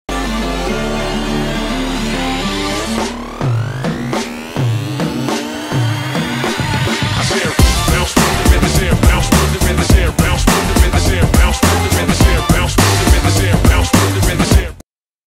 IRL Lamar Jackson walks up to a fan who is trying to take a picture with an AR Lamar Jackson
Will the real Lamar Jackson please stand up?